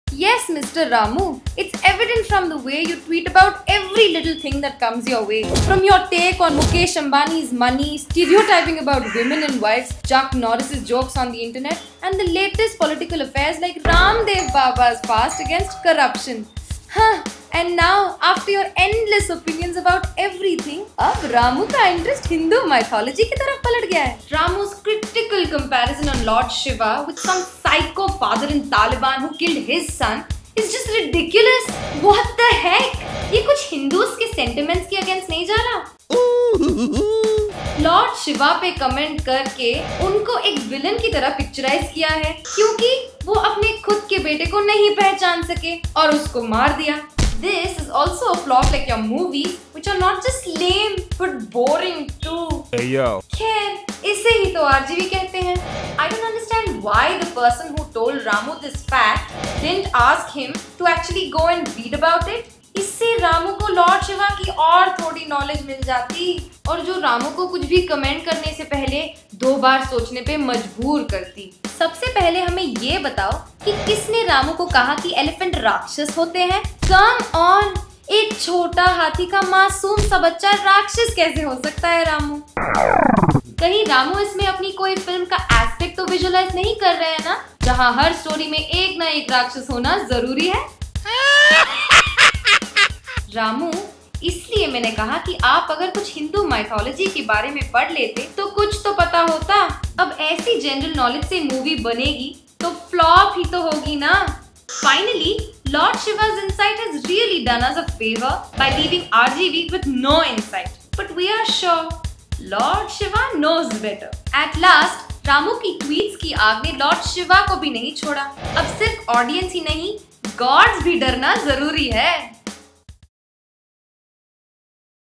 Bolly Buzz - rgv Takes on Lord Shiva
Ram gopal varma after commenting on many peoples and beliefs now takes on GOD..